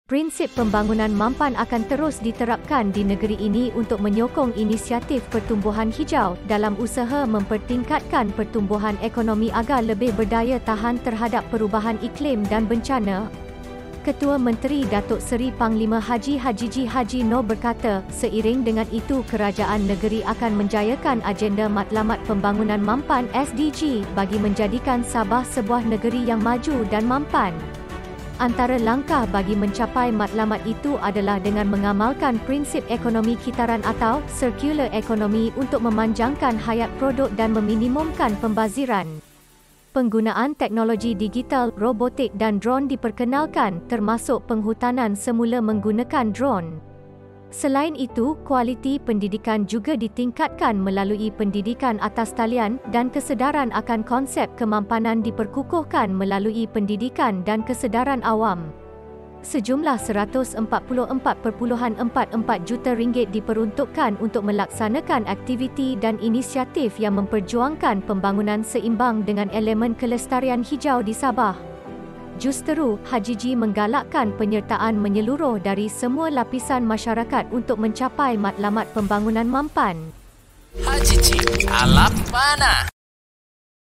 Pembangunan mampan jadi agenda utama - Hajiji
Kerajaan negeri Sabah menitikberatkan pembangunan mampan dengan penekanan pada Matlamat Pembangunan Mampan (SDG), ekonomi kitaran, teknologi digital, pendidikan dan kesedaran awam serta penyertaan menyeluruh dari semua lapisan masyarakat.